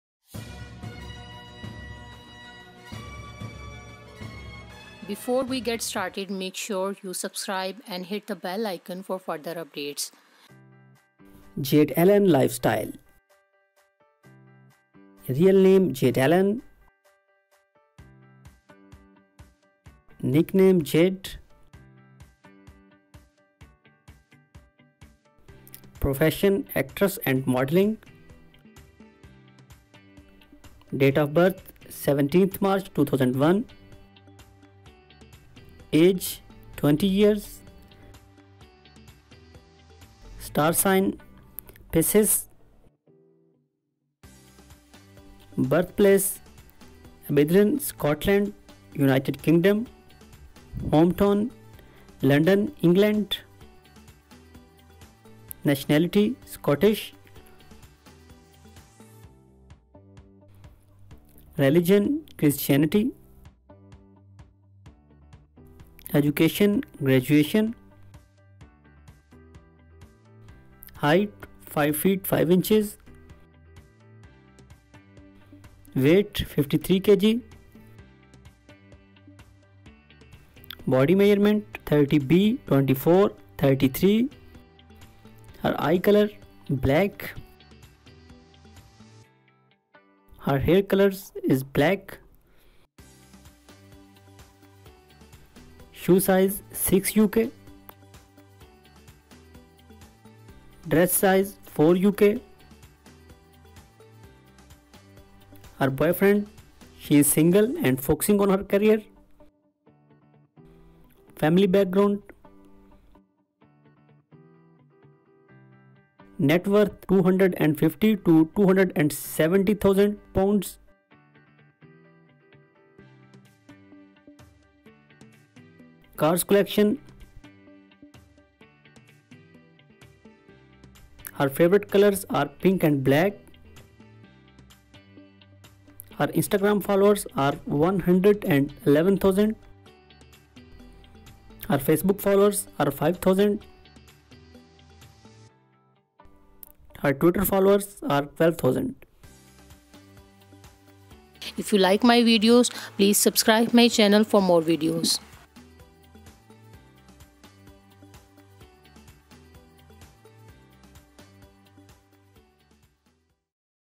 Jade Alleyne Lifestyle,Height,Weight,Age,Boyfriend,Family,Affairs,Biography,Net Worth,Salary,DOB 🔥
Jade Alleyne is a Scottish famed star. She is best known for her television roles as Tanit Ward in Netflix mystery-drama White Lines and as Clem in CBBC’s children’s comedy-drama series 4 O’Clock Club. She also played the role as Ruby Bisme-Lyons in each episode of the HBO’s science-fiction series Years and Years. When she was just 15, began her acting career. She made her acting debut with TV series The Lodge as Kaylee in 2016.

Now come to her physical texture. Her real height is 5 feet 5 inches according to 2020. Her weight is around 51-52 kilograms. She has Black hair color and eye color is also Black. Her body measurements are 32-24-33 and shoe size is 6 (UK).
Debut TV The Lodge [2016, as Kaylee]
TVs Appearance
White Lines [2020, as Tanit Ward]
Years and Years [2019, as Ruby Bisme-Lyons]
4 O’Clock Club [2015-18, as Clem]
Fan Following On Instagram 115K Followers
Fan Following On Twitter 12K Followers